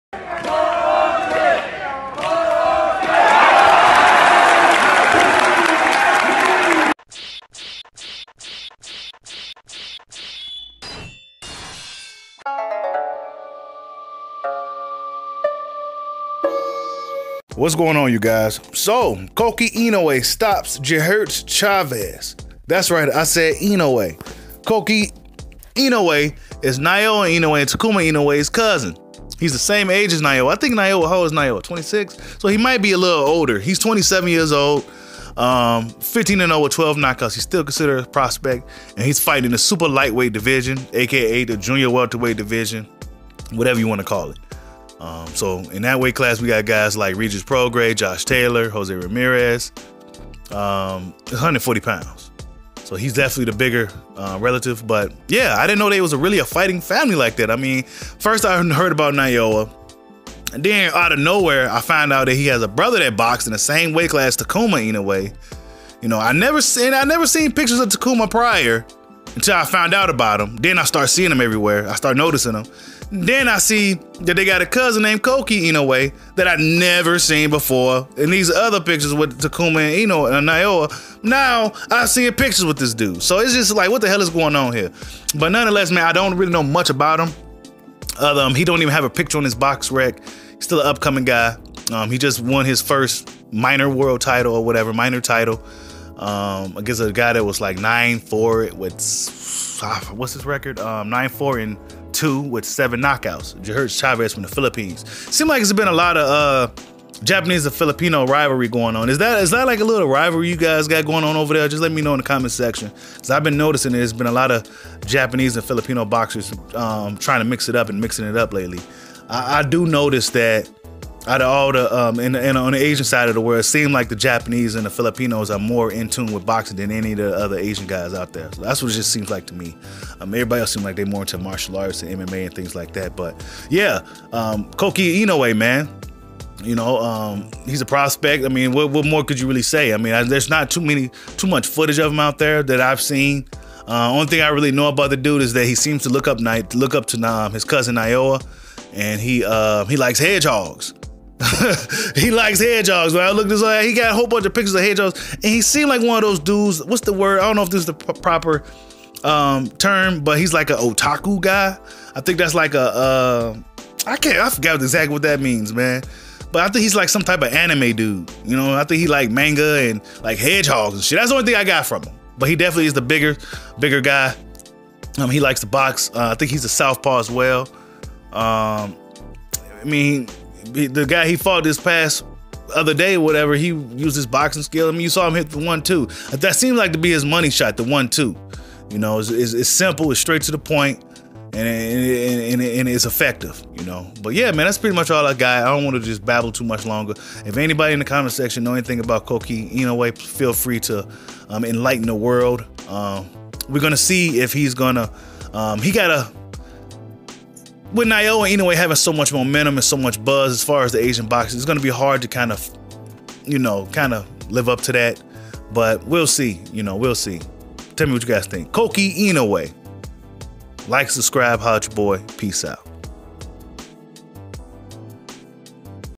The Inoue Clan 井上一族• Koki Inoue The Cousin Of Naoya and Takuma Inoue.
Koki Inoue  Naoya Inoue cousin just beat Jheritz Chavez to become WBO Asia Pacific Champion (140) He is a prospect now ranked in the Top 15(WBO).